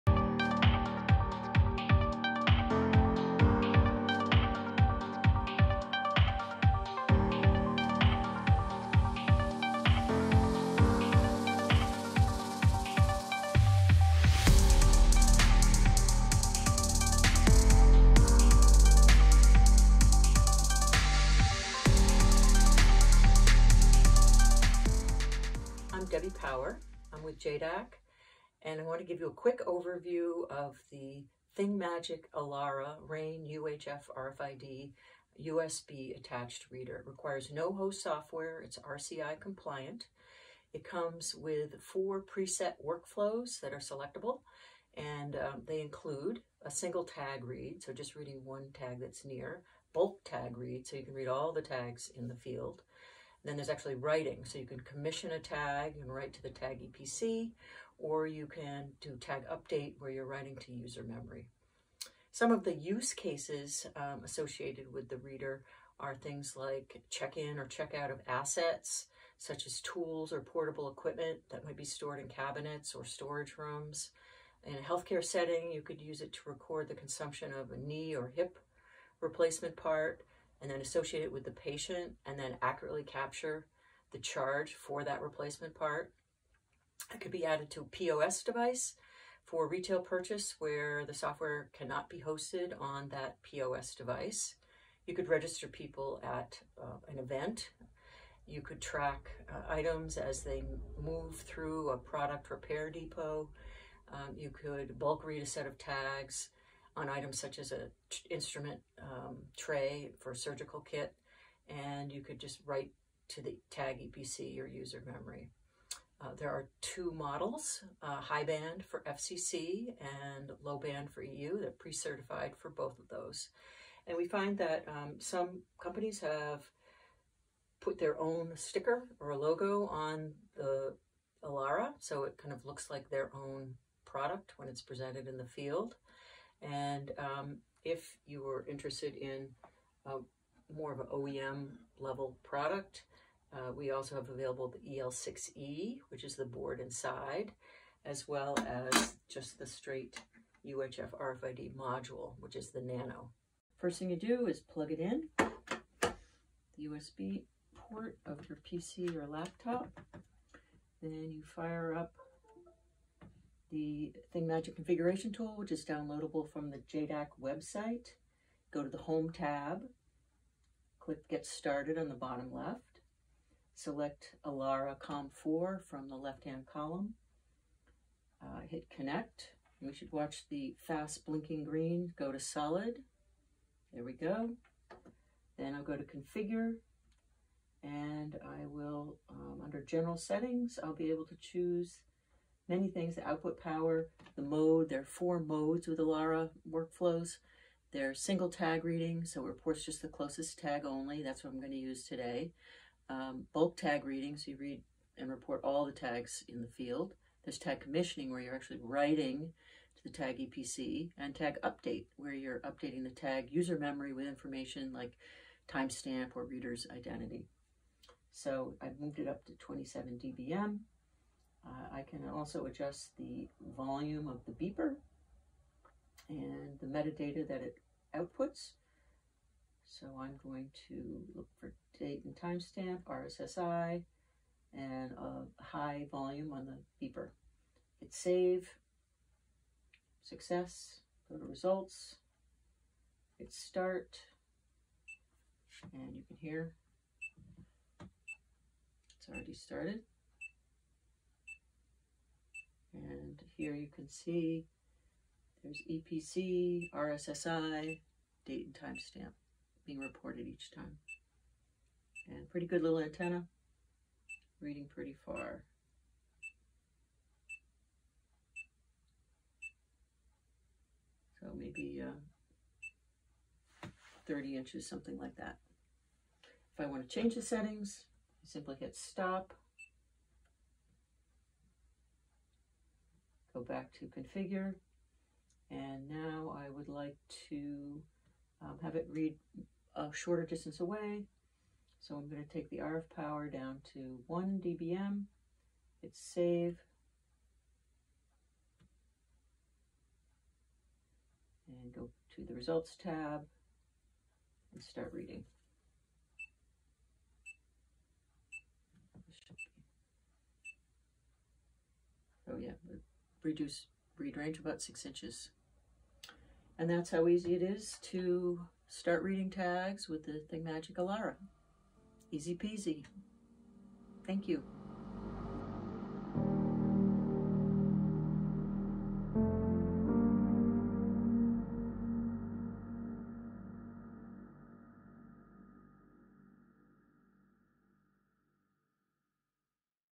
ThingMagic® Elara UHF RAIN RFID Reader Demonstration
The ThingMagic Elara (UHF) RAIN RFID Reader is an all-in-one plug-and-play USB RFID scanner for short and medium range applications that require a quick setup and easy customization. See it in action.